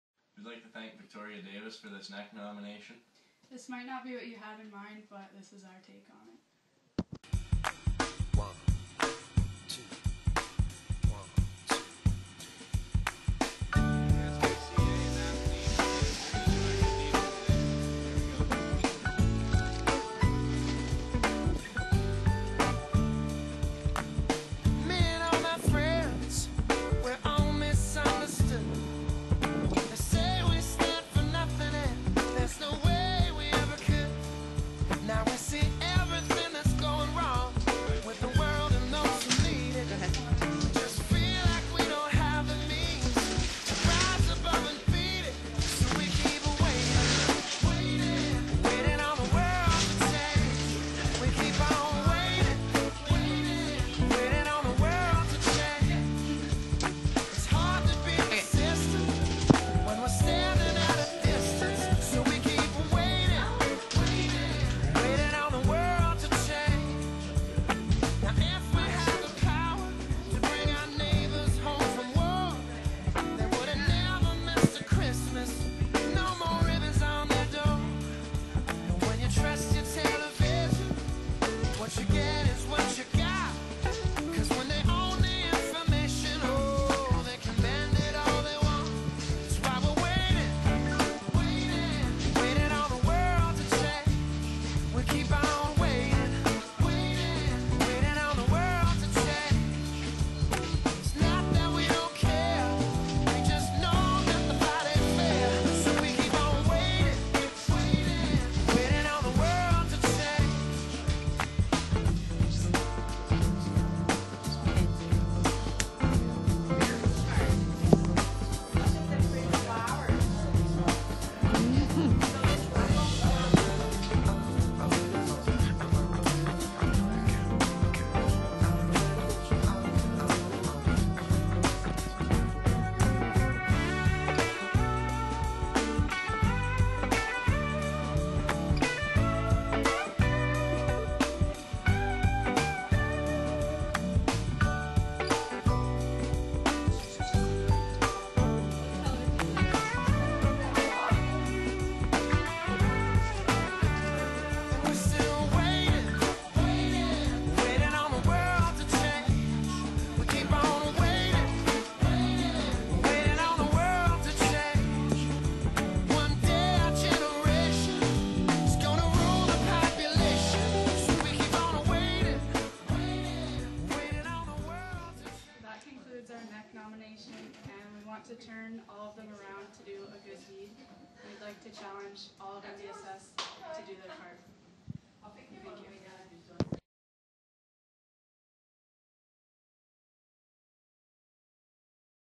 Neknomination! Lets put a smile on someone elses face.
We are 3 students from Napanee District Secondary School who got a Neknomination. This is our spin on it.